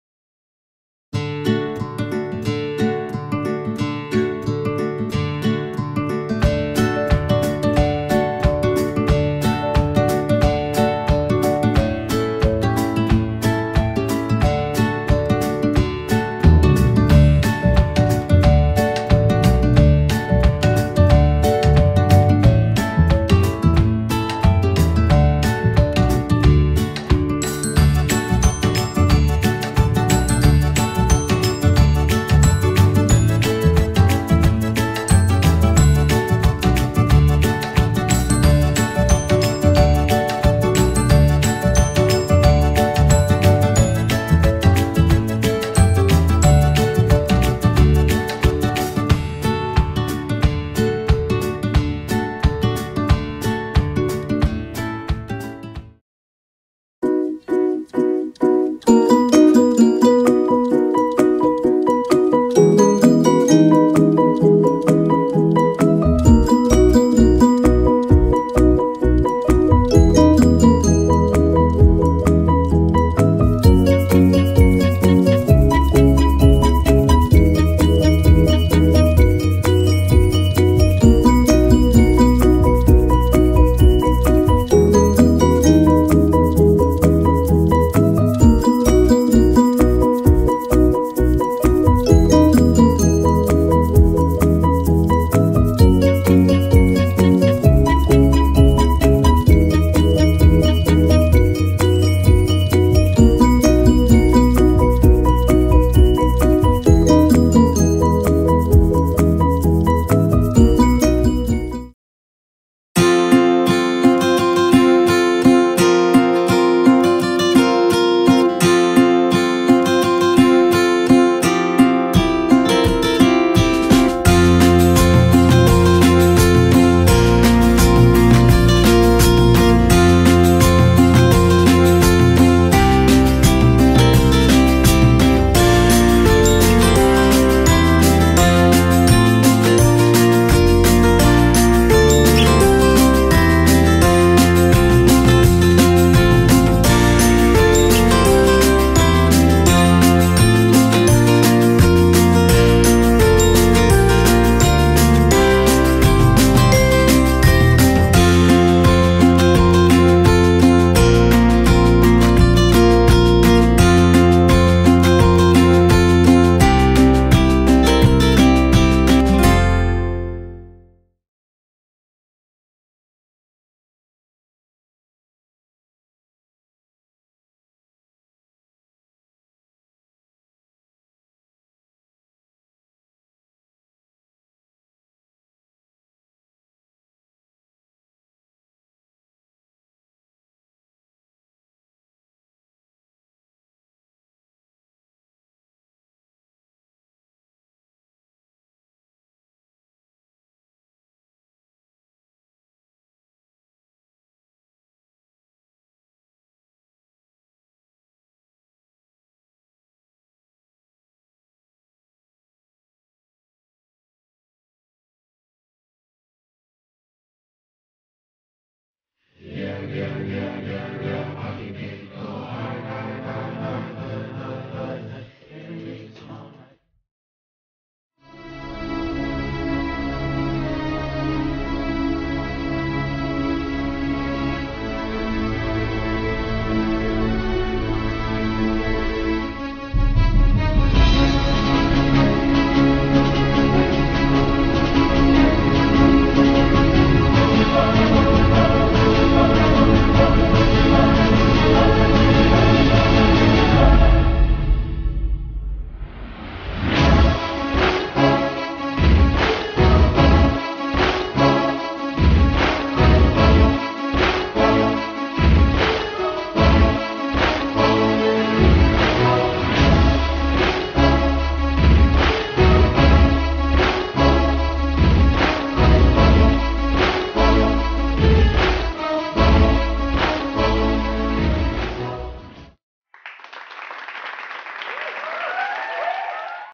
2015 Year 8 Presentation
Homebush Boys High School
2015 - Year 8 Students Photo Presenatation